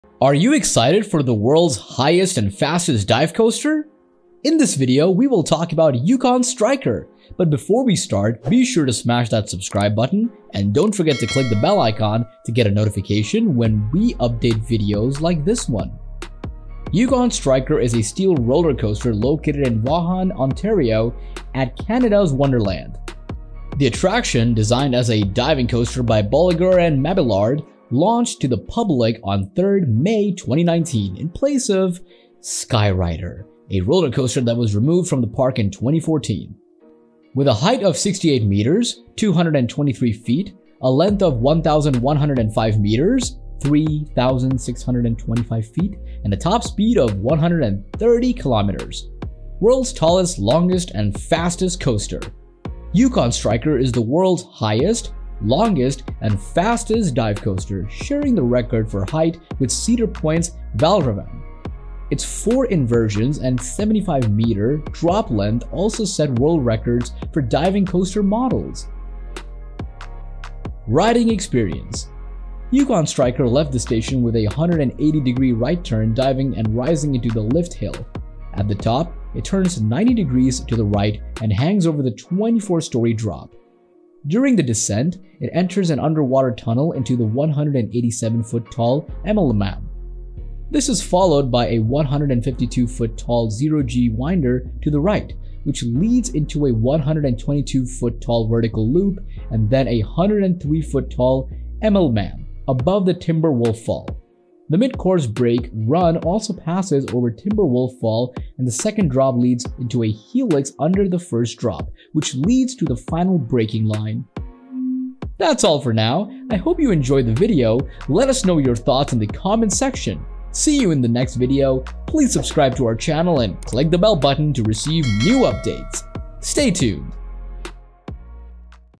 YUKON STRIKER | Fastest & Tallest DIVE COASTER | CANADA'S WONDERLAND
The YUKON STRIKER is the world's FASTESTS, LONGEST, and TALLEST DIVE roller COASTER, reaching speeds of up to 130 km/h (81 mph) and featuring a drop of 75 metres (246 ft). The ride is 4,124 feet in length, making it the longest roller coaster in the world. The Yukon Striker is a roller coaster located at Canada's Wonderland amusement park in Vaughan, Ontario, Canada.

Yukon Striker is a Bolliger & Mabillard dive coaster which is the first of its kind in Canada, and the third in the world. The ride features a 90-degree drop, which is the steepest drop in the world. The ride also features a "hold and dive" element, in which the train slows down and pauses at the top of the drop before plummeting downwards. The ride also has four inversions, including a barrel roll and a 360-degree loop.

The ride is themed around the Klondike Gold Rush of the late 19th century, with the train resembling a steam-powered gold-mining train. The station is designed to look like a gold-mining town, complete with wooden buildings and a working water wheel.

Yukon Striker has received widespread acclaim from roller coaster enthusiasts and critics, with many praising its intense ride experience and immersing theming. It has also been named one of the top roller coasters in the world by several publications and theme park websites.